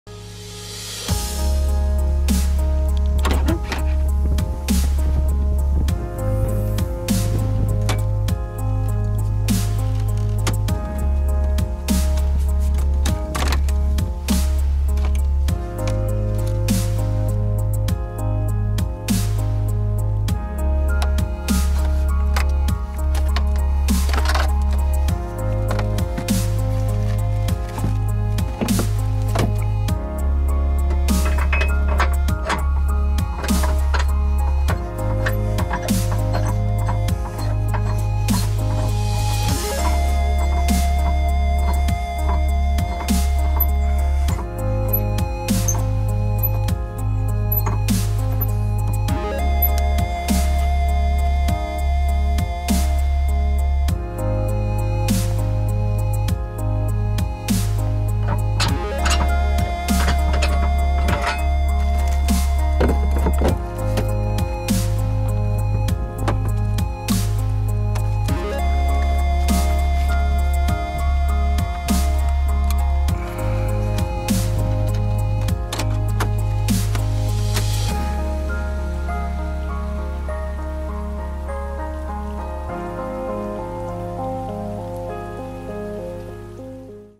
Tesla Model S -Nosecone Bombrikke Slepekrok
Hvordan åpne Nosecone på en Tesla Model S, montere bombrikke og slepekrok
How to open the nosecone on a 2014 Tesla Model S, mounting the tow hook,